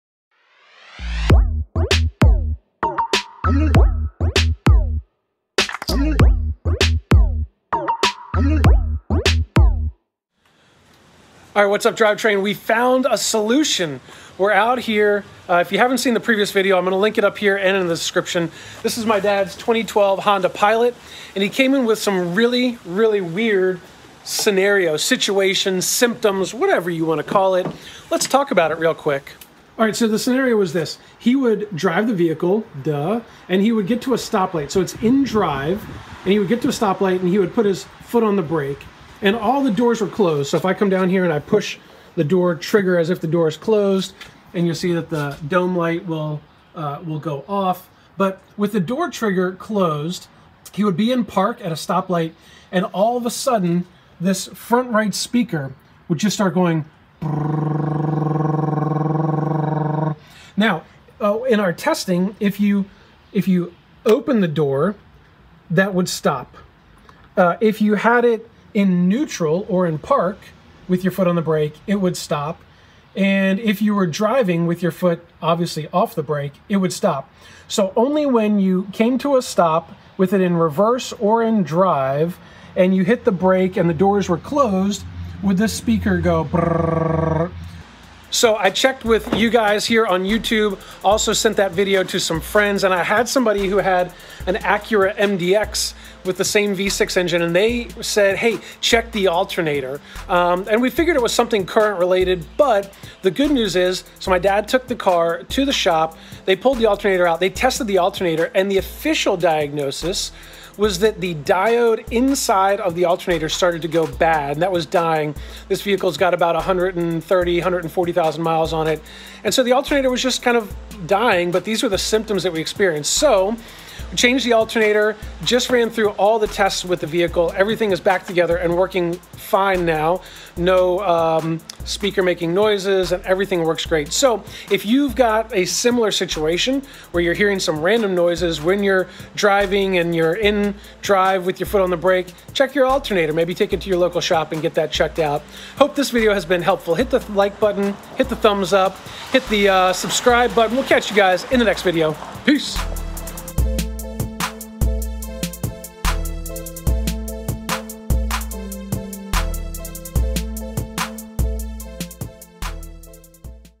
Solution: Weird Honda Electrical Issue
In a previous video we discussed an issue my dad was having in his 2012 Honda Pilot. We found a solution and determined the diode in the alternator was going bad and the alternator needed to be replaced. Once the alternator was replaced, we reinstalled the front speaker and went through all of the testing again. The great news is that the sounds are gone and everything works properly now.